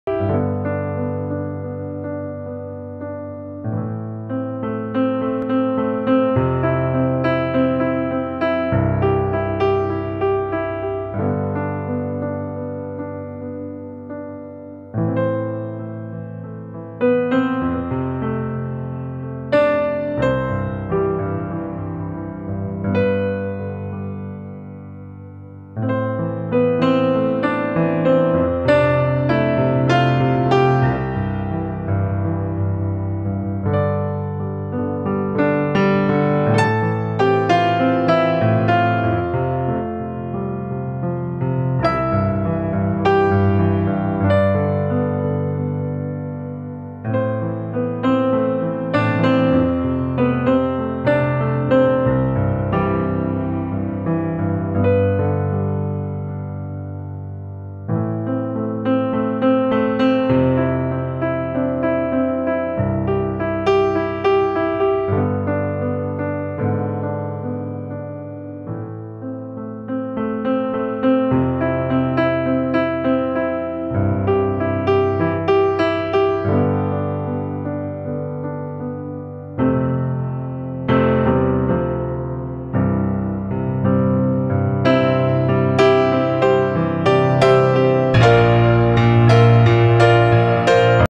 Hologram video 3d Butterfly 4k 2017 video [Mr Plash]
Hologram video 3d for pyramid-Transform your phone in a 3d projector just using a plastic pyramid